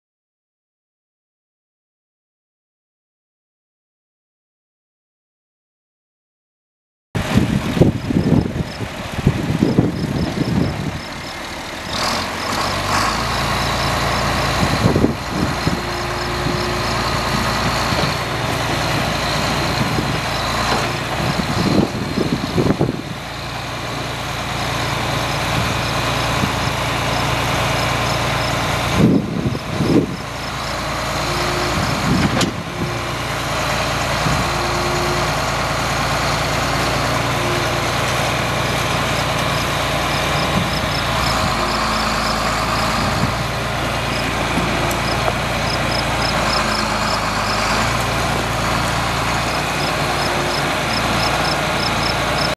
1982 John Deere 4640 tractor for sale | sold at auction July 11, 2012
1982 John Deere 4640 tractor
9,274 hours on meter
John Deere six cylinder twin turbo diesel engine
Serial 6466AR-07202526RG
Manual transmission
Differential lock
16F-8R gears
AC and heat
AC non-operational
AM/FM
Tilt steering wheel
Front windshield wipers
Suspension seat
Front and rear work lights
Three point hitch
Draw bar
(3) sets of rear hydraulic remotes
Westendorf mechanical self leveling loader
Quick attach loader
97" grapple bucket
(4) tines
(3) tines are bent

1000 PTO
14L-15.1 front tires
18.4R42 dual rear tires
Replaced starter and front spindle two years ago
Door hinge is broken
Serial 4640H027777RW
 Bid for Blue program asset

It was located in El Dorado, KS, and we sold it to the highest bidder for $17,600.